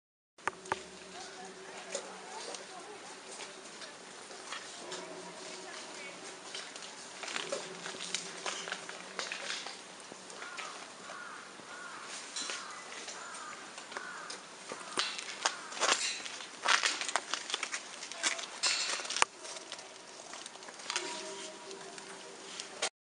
１２月１０日のイコロ
短いですが、氷のバースデーケーキの上の段を食べているところです。もう少し
写真や動画を撮れる余裕がほしい・・